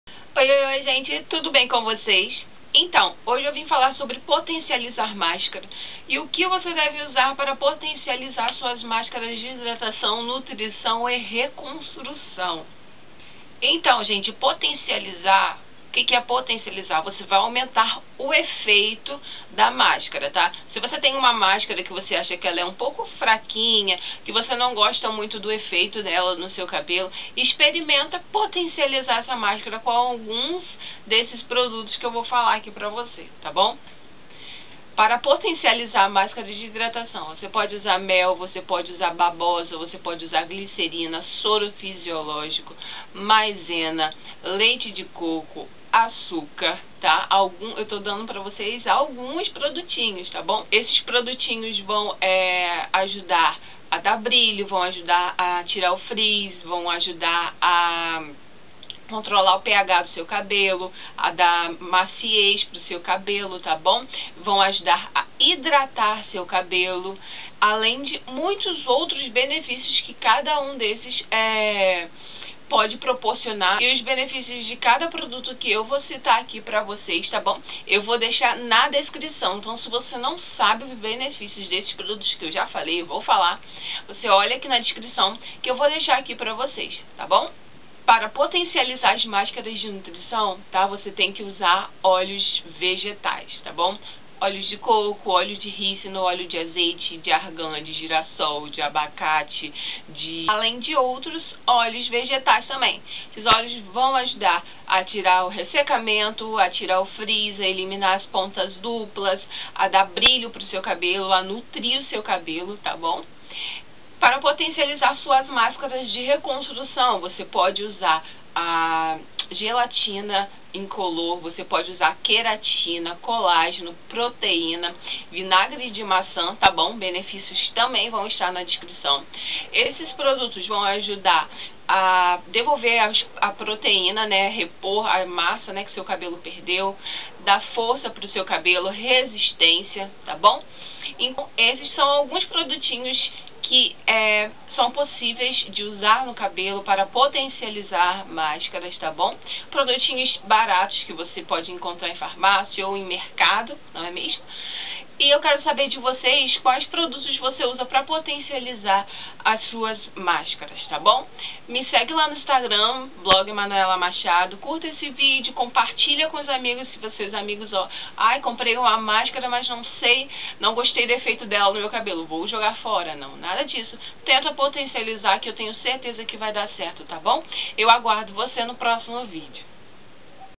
Como aumentar o efeito das máscara | Canal Manoela Machado
Oi gente tudo bem, você sabe como aumentar o efeito da máscara? No vídeo de hoje eu vim falar sobre potencializar máscara, o que você deve usar para aumentar o efeito das suas máscaras de hidratação, nutrição e reconstrução.

O que usar na hidratação?
- Mel: Hidrata, tira o ressecamento, da brilho, diminui o frizz, ajuda no crescimento e ajuda a combater as pontas duplas.
- Babosa (aloe vera): Ajuda a combater a queda e no crescimento, hidrata, nutri a fibra capilar e combate a caspa.
- Glicerina: Promove hidratação, brilho e maciez, controla o frizz, combate as ponas duplas e estimula a definição dos cachos.
- Soro fisiológico: Equilibra o ph, da brilho e maciez, deixa o cabelo mais solto, combate o ressecamento  e pontas duplas.
- Maizena: Ajuda a combater a oleosidade, alinha os fios, contra frizz e ressecamento.
- Leite de coco: Ajuda a controlar a aqueda e a restaura os fios, funciona como condicionador natural, combate ressecamento e nutri os fios. (pode ser usado na etapa de nutrição)
- Açúcar: Promove brilho, maciez, alinhamento e diminui o frizz além funcionar como  esfoliante capilar

O que usar na nutrição?
Óleos vegetais: Combate: frizz, ressecamento e pontas duplas, devolve nutrientes ao cabelo, ajuda nos crescimento, fortalece e encorpa os fios.

O que usar na reconstrução?

- Gelatina incolor: Promove maciez, fortalecimento, hidrata e recupera elasticidade.
- Queratina: Fortalecimento capilar, resistência, força e alinhamento.
- Colágeno: Volume, fortalecimento, brilho, hidrata e previne queda.
- Vinagre de maçã: Ajuda a fechar a cutícula, da brilho e reduz o volume.

(Só citei alguns benefícios)

Ultimo vídeo a diferença entre: Hidratação, nutrição e reconstrução capilar. Assista depois ele para entender esse.